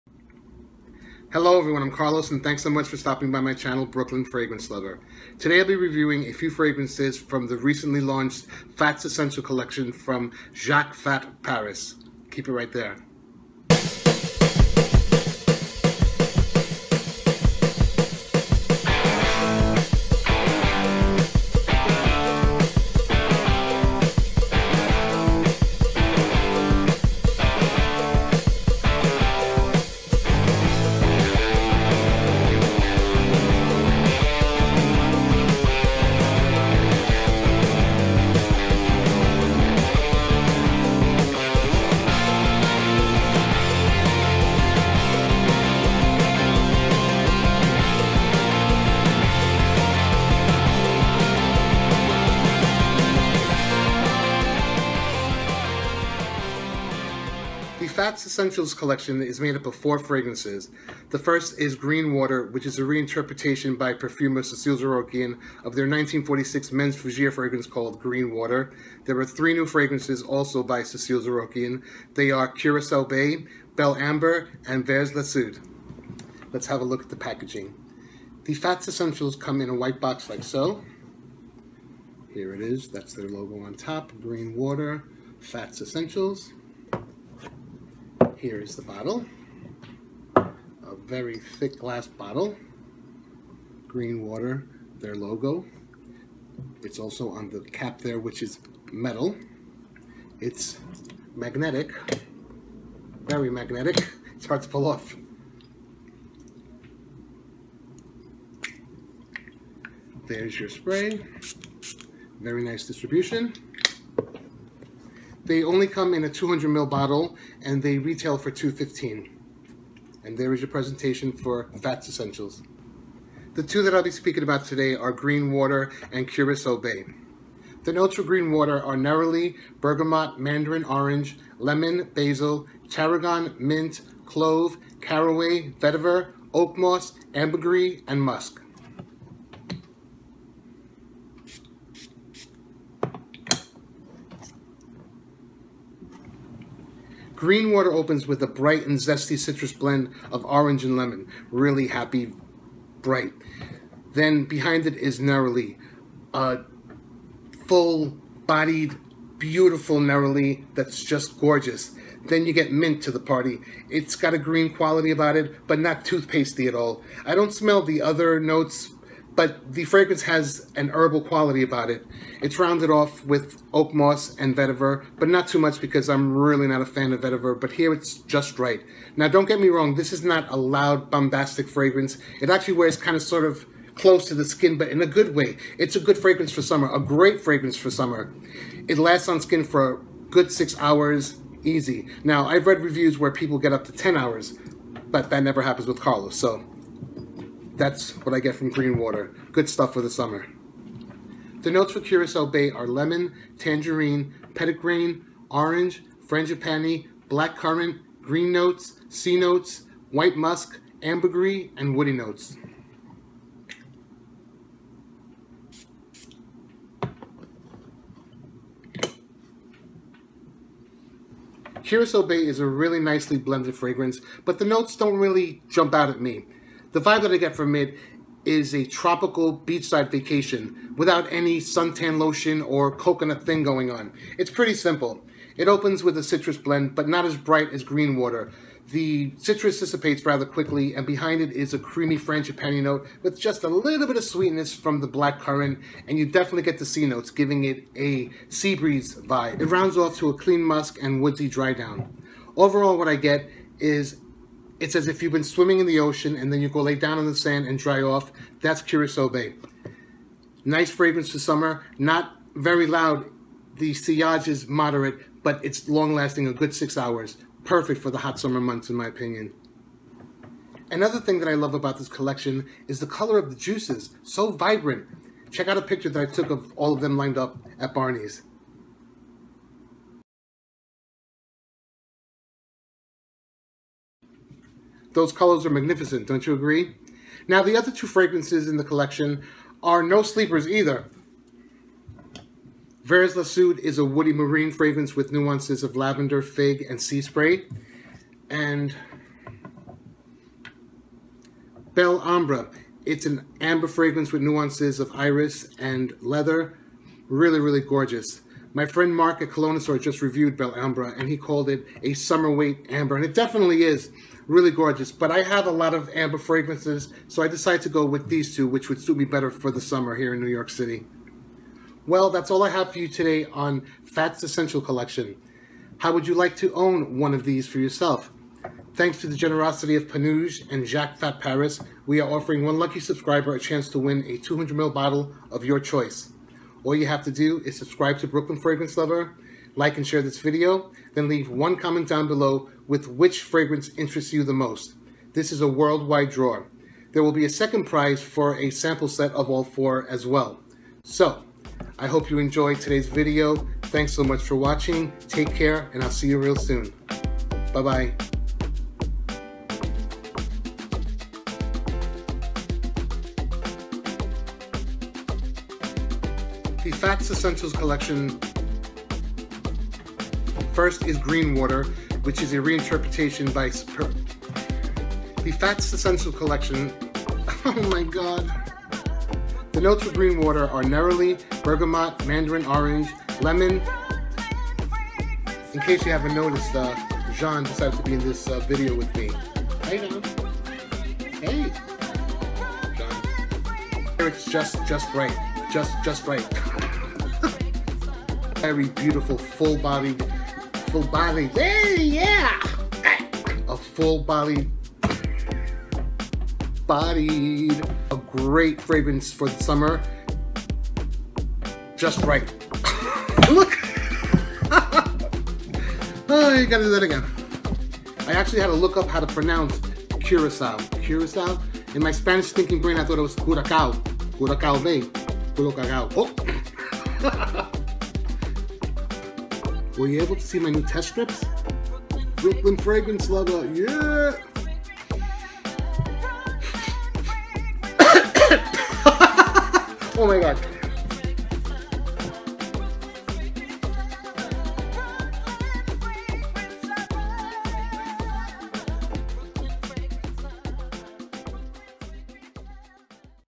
NEW Jacques Fath Paris Fath's Essentials Collection REVIEW + Full Bottle GIVEAWAY (CLOSED)
YOU CAN CHOOSE FROM ALL FOUR, NOT JUST THE TWO FEATURED IN THE VIDEO.